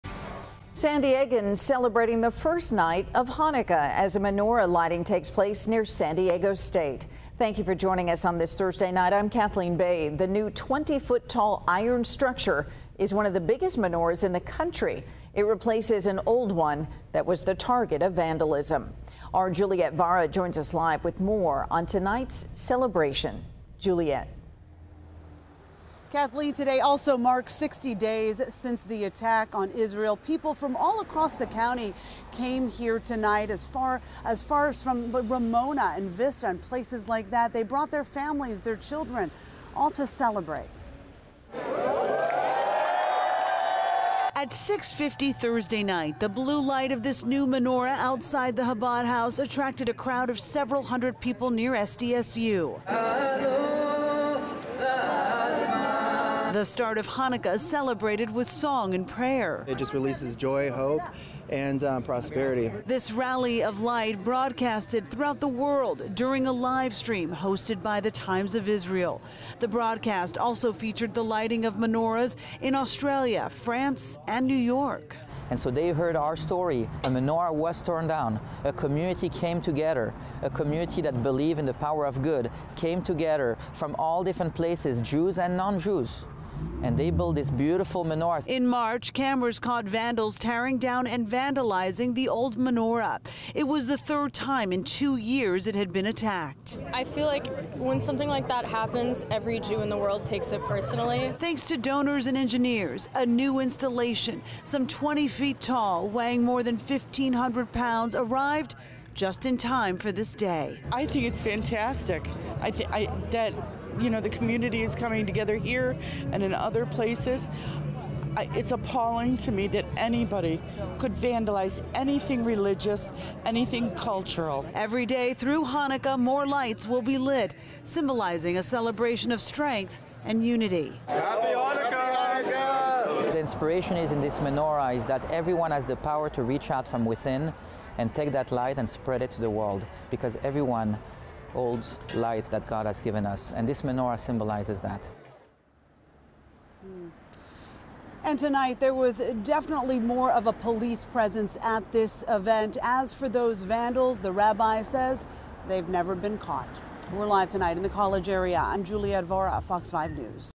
Hanukkah Menorah Lighting Celebration
Juliette Vara reports.